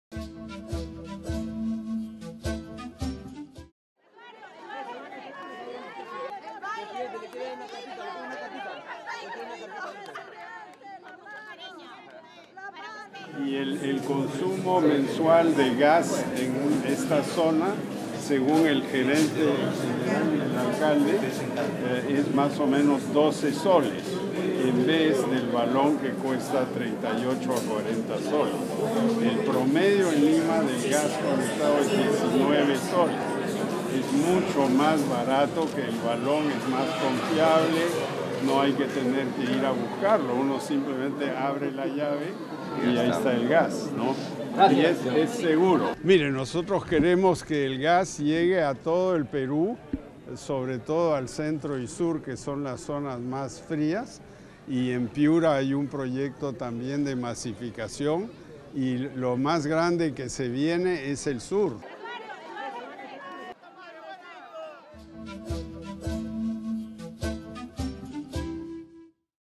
Avances de la masificación del Gas Natural Domiciliario
Presidente de la República, Pedro Pablo Kuczynski, supervisa el proceso de masificación del Gas Natural domiciliario, realizando una conexión simbólica en el comedor "María Parado de Bellido" en el distrito de Comas.